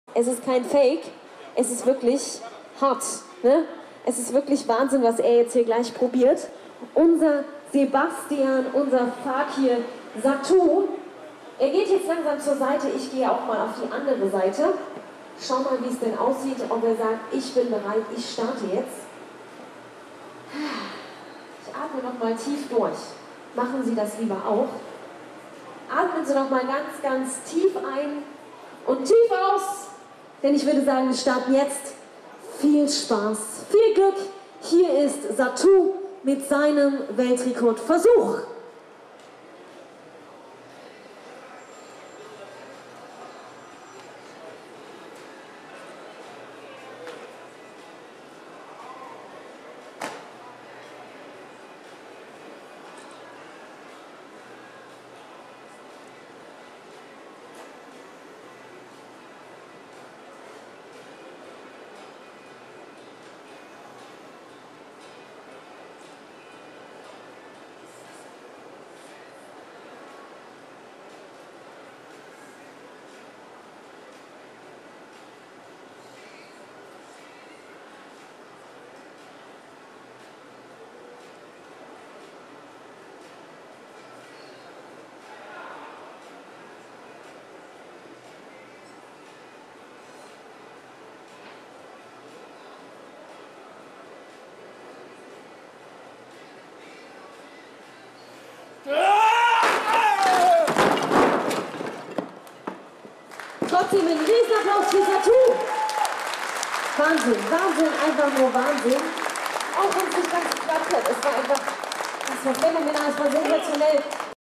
30 Steine mit Kopf zerschlagen! Krasser Weltrekord-Versuch von Sebastian SATU Kopke
BITTE NICHT NACHMACHEN! Sebastian SATU Kopke (bekannt aus "Wetten, dass..?" oder "Das Supertalent") startete auf den Berliner Motorrad Tagen 2016 einen spektakulären Weltrekord-Versuch. 30 Gasbetonsteine wollte er mit dem Kopf zerschlagen - und scheiterte zunächst. 24 Stunden später hat es übrigens geklappt...